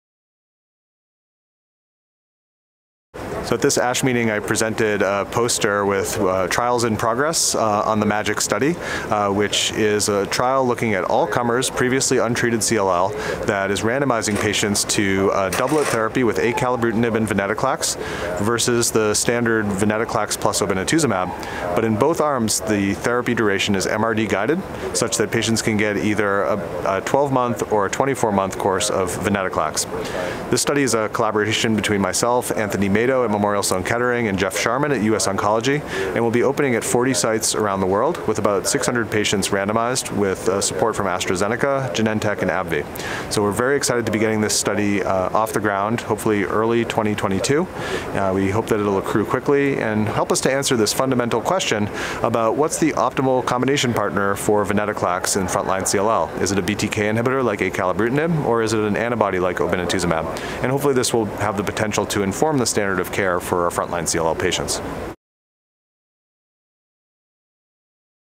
MAJIC: Venetoclax with acalabrutinib or obinutuzumab in CLL
Matthew Davids, MD, Dana-Farber Cancer Institute, Boston, MA, discusses the trial design of the open-label, randomized Phase III MAJIC trial (NCT05057494), which will investigate acalabrutinib, a Bruton tyrosine kinase (BTK) inhibitor, with venetoclax versus obinutuzumab with venetoclax in patients with relapsed/refactory chronic lymphocytic leukemia (CLL). Therapy duration in both arms will be based on minimal residual disease (MRD) status, where patients will either receive 12 or 24 months of venetoclax. This trial will aim to assess whether BTK inhibitors or antibodies are the best approach for CLL. This interview took place at the 63rd ASH Annual Meeting and Exposition congress in Atlanta, GA.